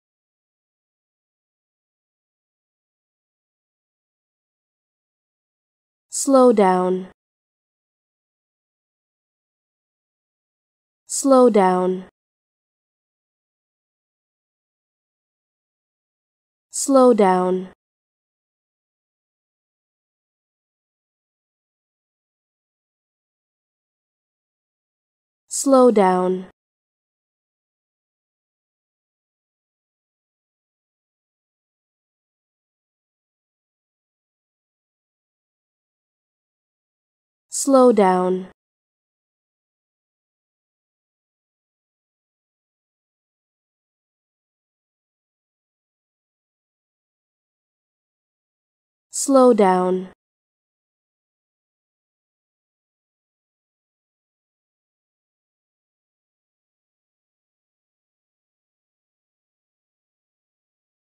How To Say Slowdown
Learn how to say Slowdown with EmmaSaying free pronunciation tutorials.